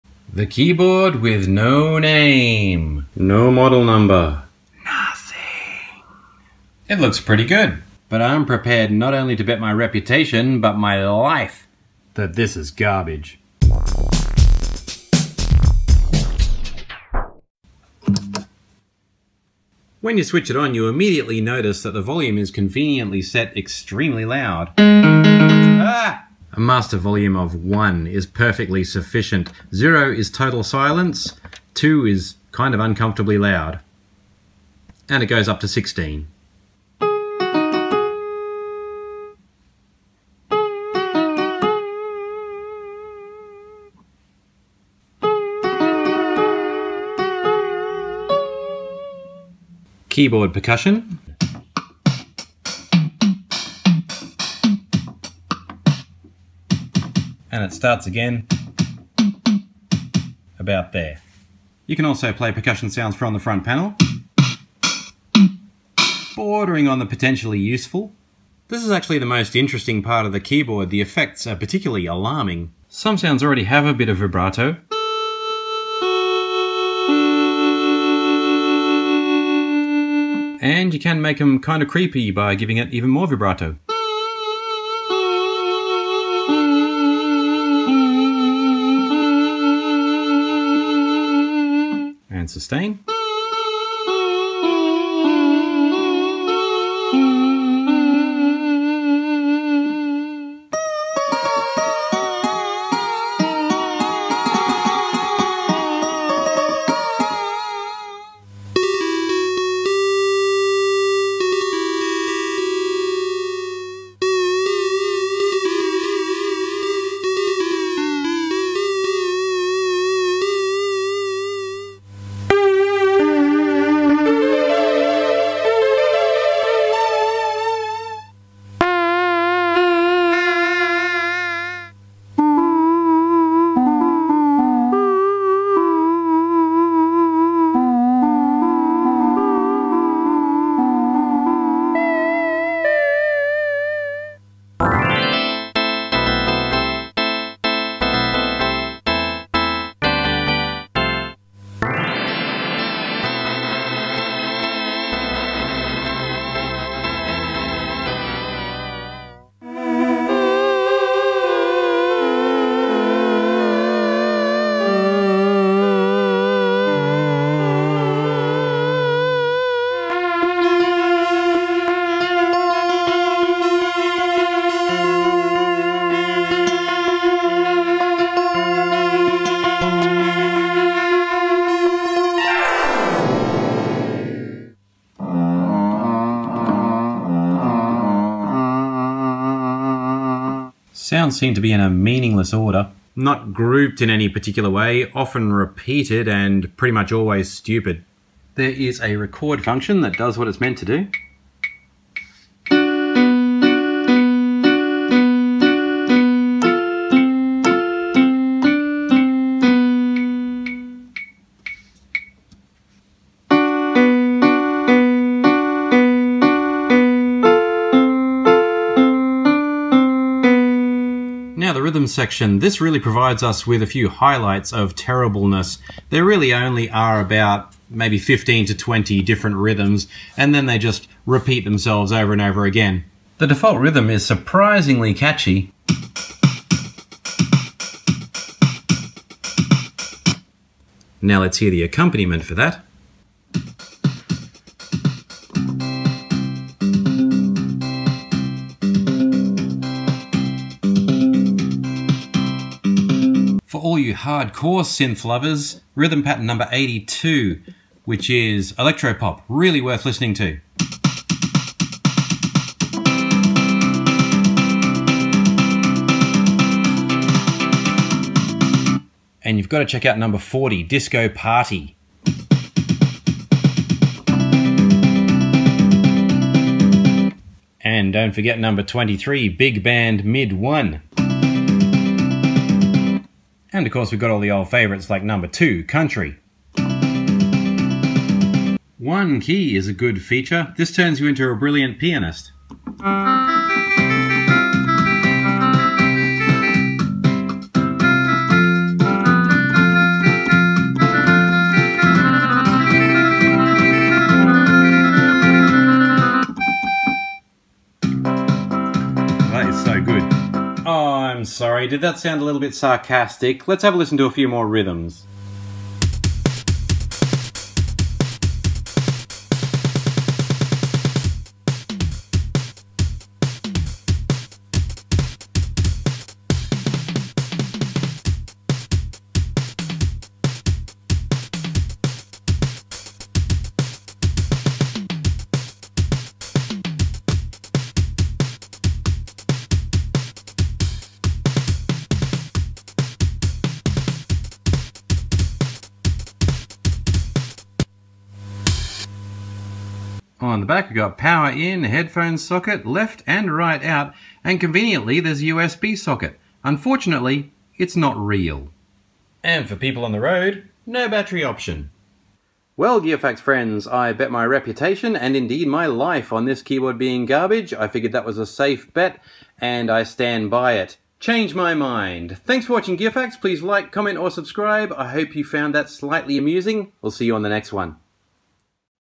Mystery keyboard with no name has some scary surprises
I mocked this mysterious unbranded keyboard during the review, but listening over it again I can see that there is actually a personality there, and some quite interesting sounds ...but it's essentially garbage :)